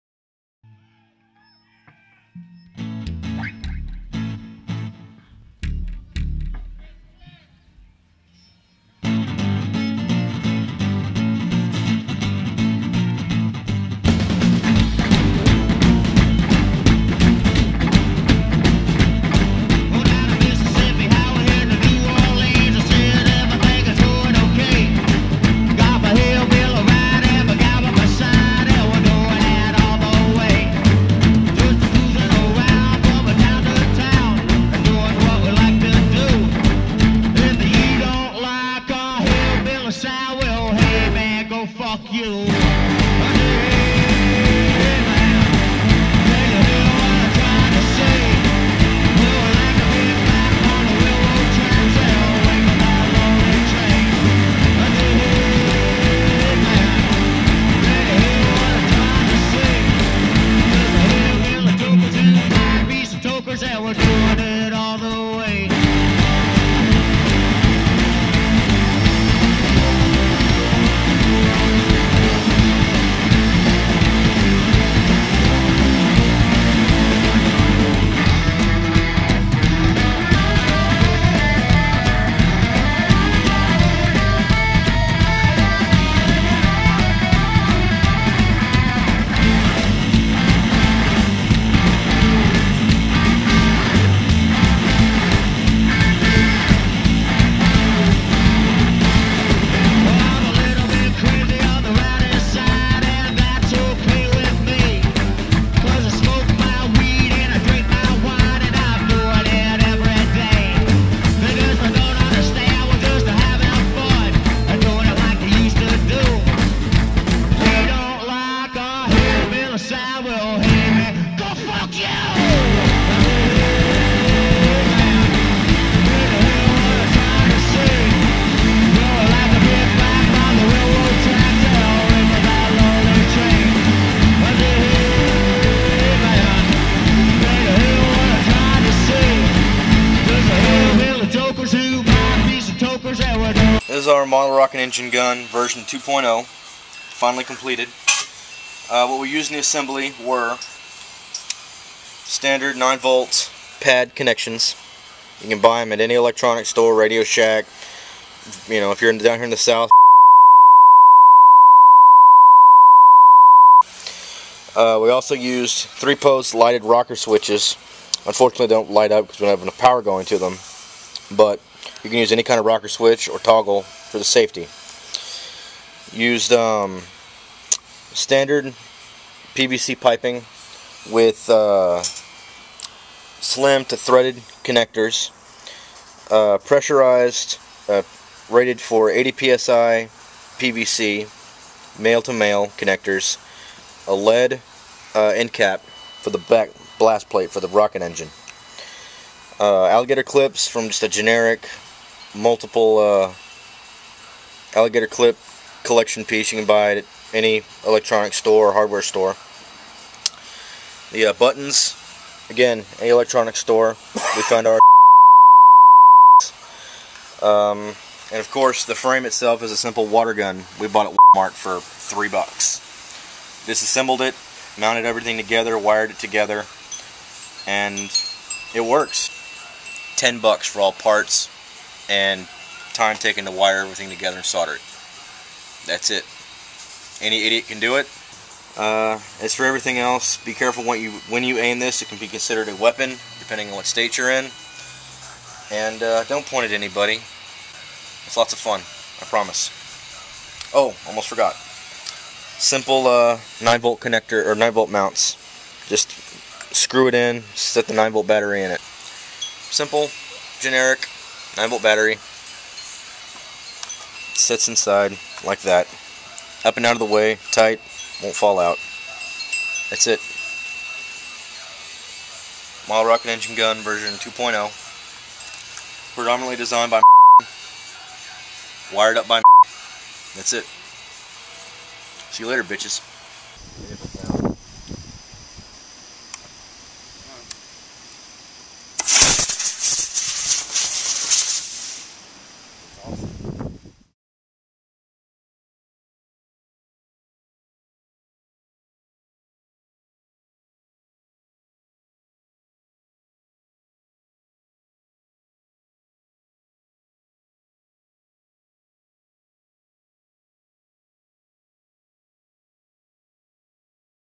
Model Rocket Engine Gun 2.0
the boys decided they could improve the model rocket engine gun. so they set out to do it as a team. abomb and scribbles designed and wired up a helluva gun. double barreled action.